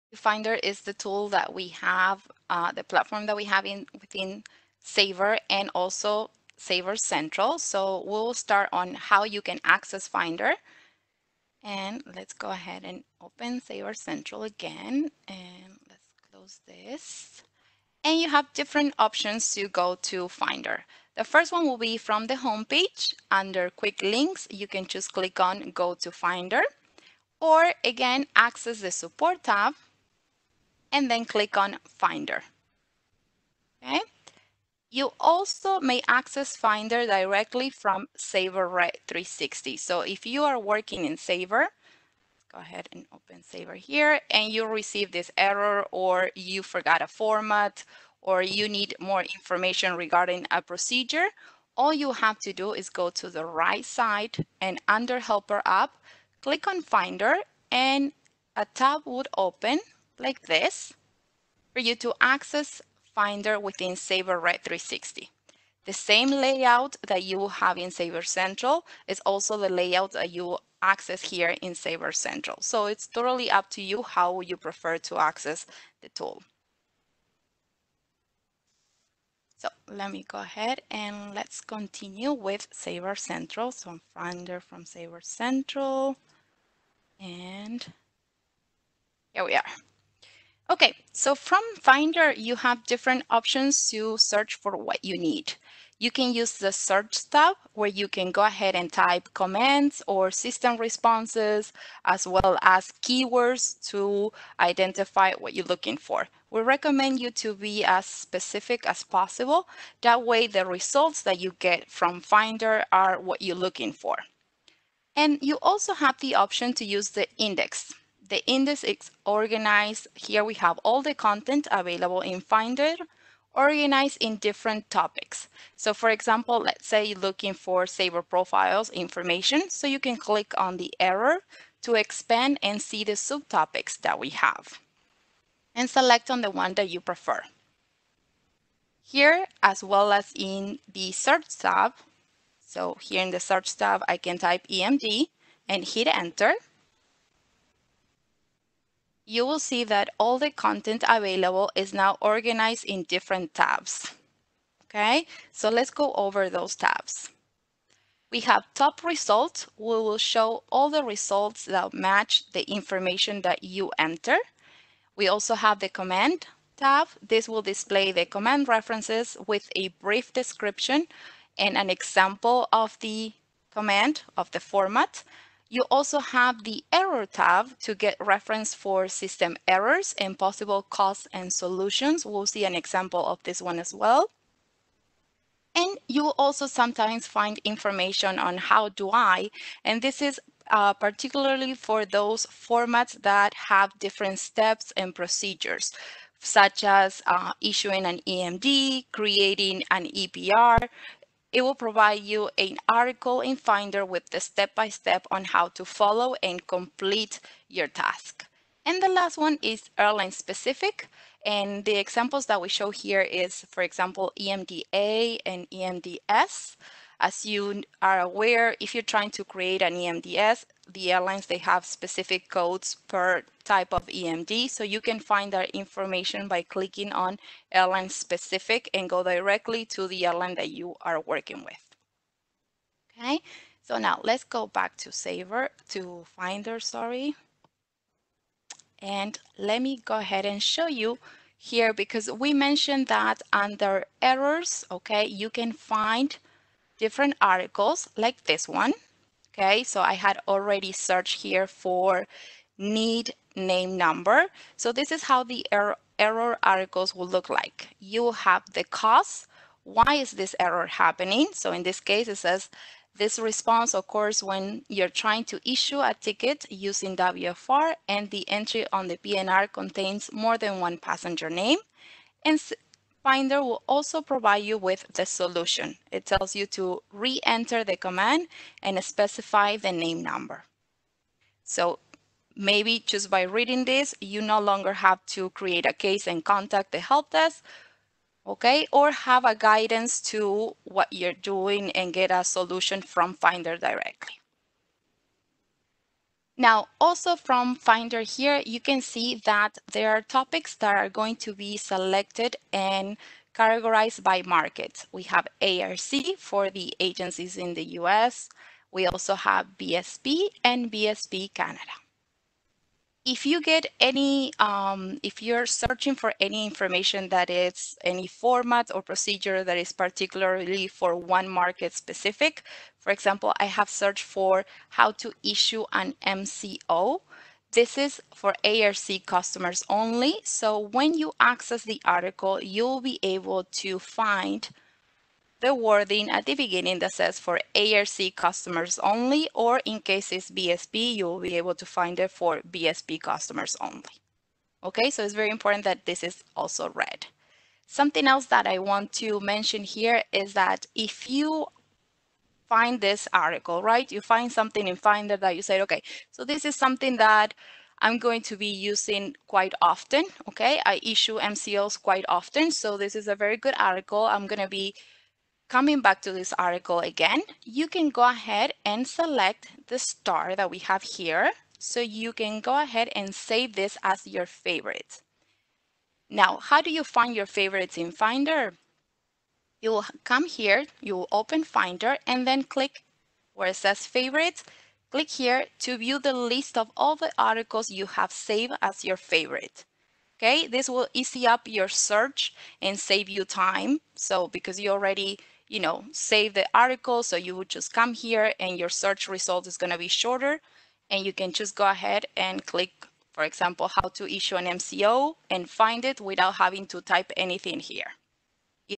Finder | Formats searching tool
Find all the formats  you need to maximize  your Sabre Red 360 experience in one single place: Finder.